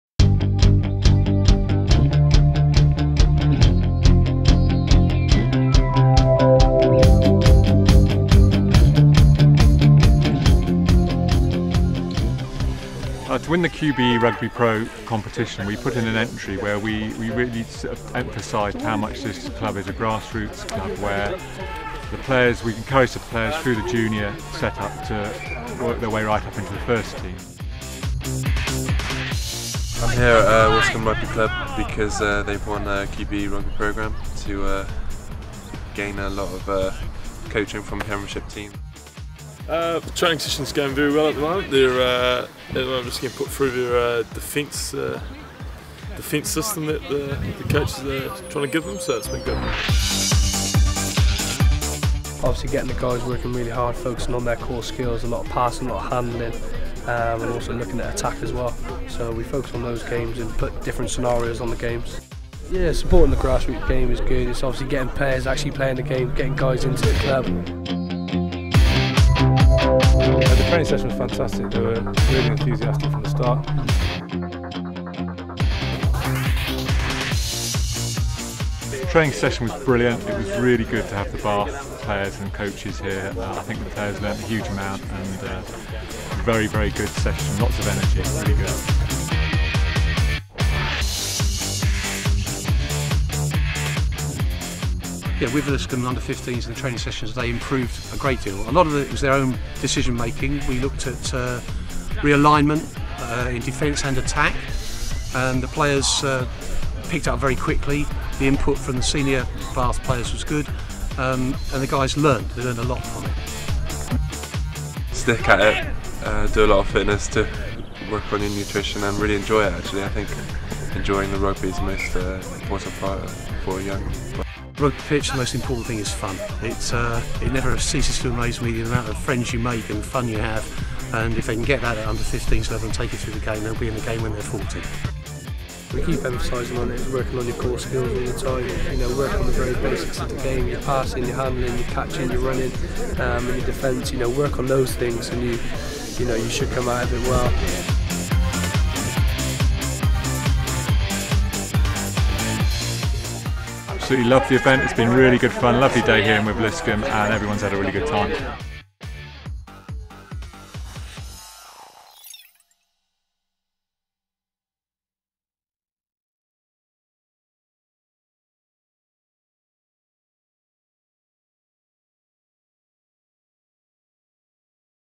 QBE Rugby Pro Campaign 2012 | Premiership Rugby
Since 2009, QBE has worked with all levels of the game to promote the sport and our brand.

The initiatives that we run are as wide ranging as sponsoring England Rugby to running Rugby Pro, our competition to win training sessions and £1000 of kit for your local rugby club.